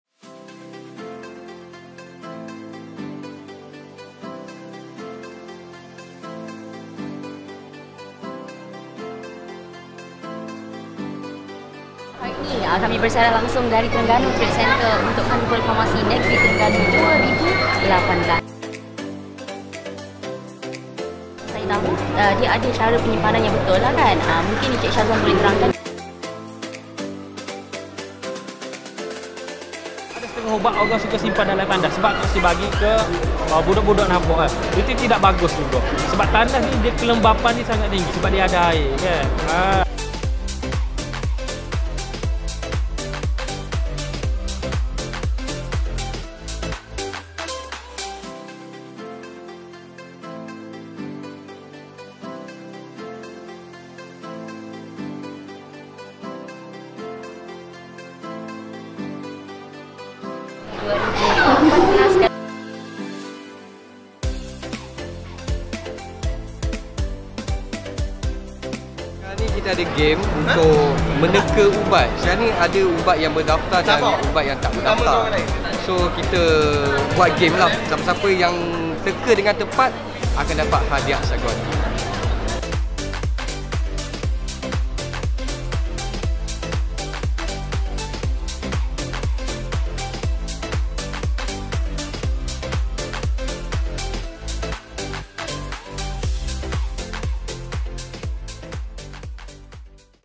Siaran Luar Manis FM bersama Jun Hamid di Karnival Farmasi Negeri Terengganu 2018
Siaran Luar Manis FM bersama Jun Hamid di Karnival Farmasi Negeri Terengganu 2018 di Terengganu Trade Centre (Galeri B) .